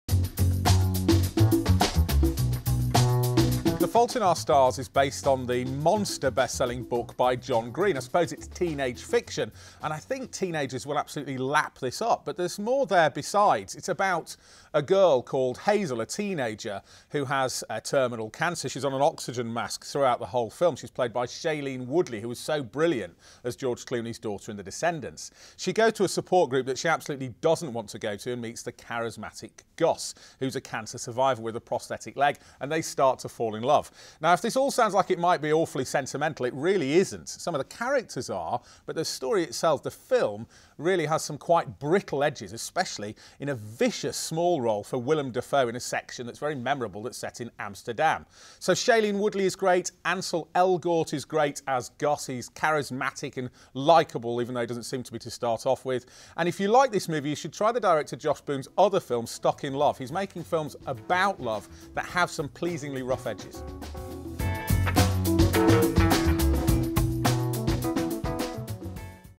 The Fault in our Stars Movie Review
Film critic Nick Duncalf reviews the movie adaptation of John Green's blockbuster young adult novel The Fault in Our Stars, for 60sec Movies. Teenagers Hazel and Gus meet at a cancer support group and soon fall in love.

Full Cast List for The Fault in our Stars:
Shailene Woodley
Ansel Elgort
Nat Wolff
Laura Dern
Sam Trammell
Willem Dafoe
Lotte Verbeek
Ana Dela Cruz
Randy Kovitz
Toni Saladna
David Whalen
Milica Govich
Allegra Carpenter
Emily Peachey
Emily Bach